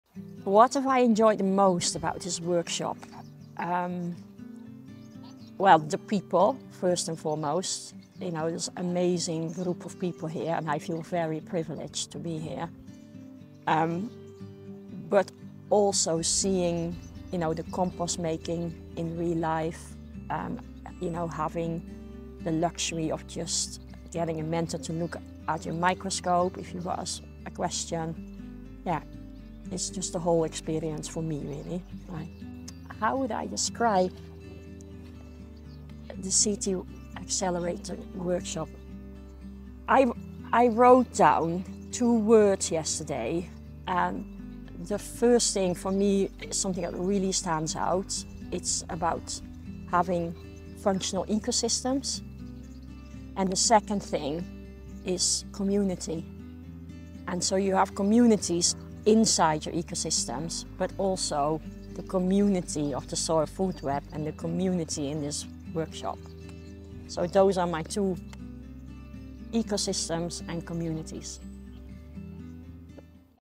What makes a Soil Food Web School workshop unforgettable? 🌱
For Anuschka, it’s the people—and the magic of seeing concepts like compost making come alive.

She summed it up perfectly with two words: functional ecosystems and community.